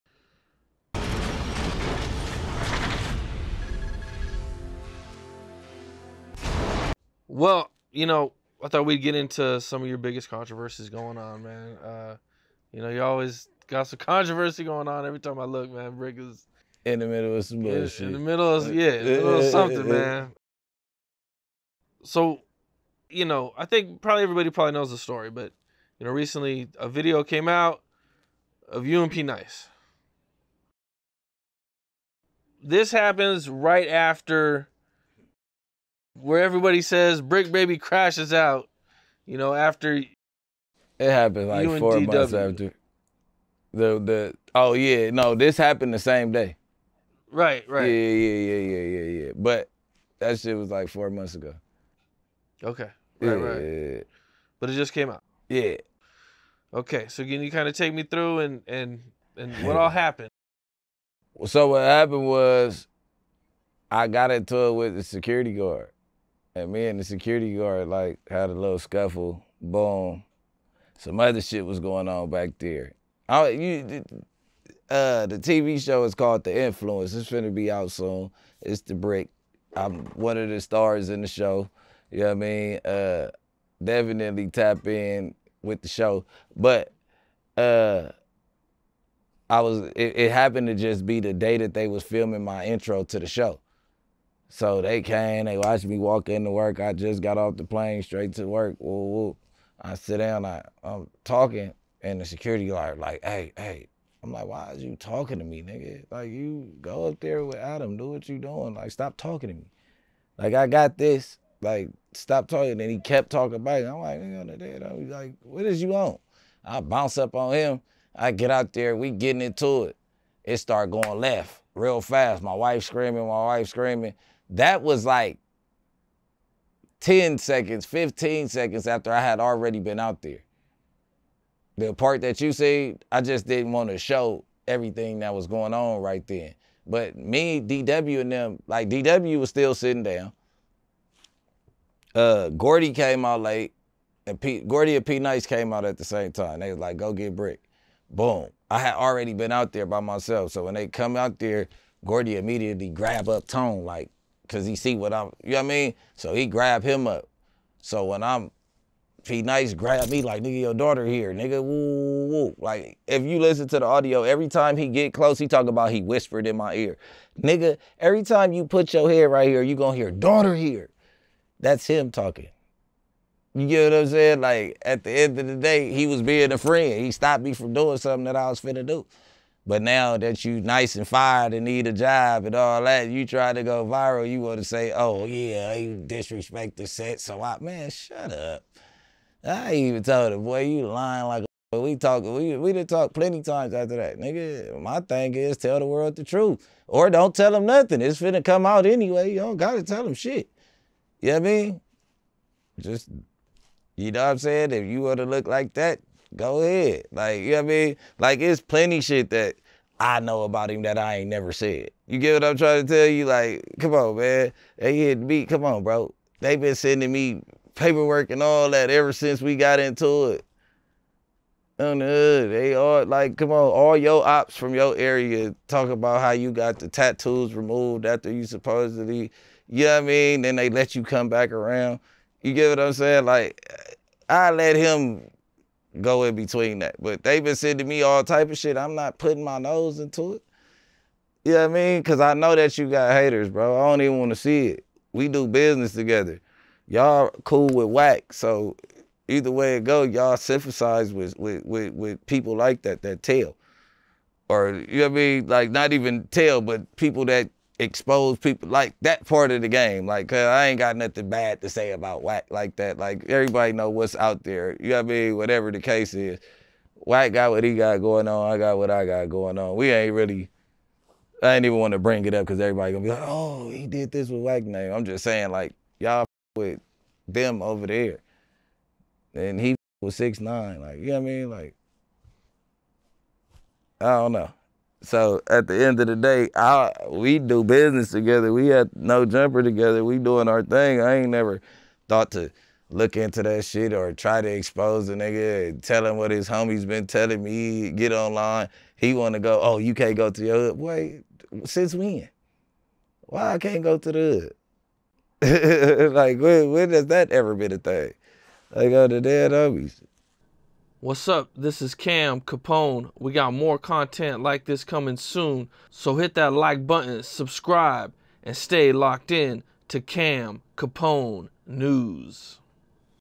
Bricc Baby Breaks Down His Situation w/ P Nice
PART 5
See The Full & Uncensored Interview Without Any Ads Before It's Released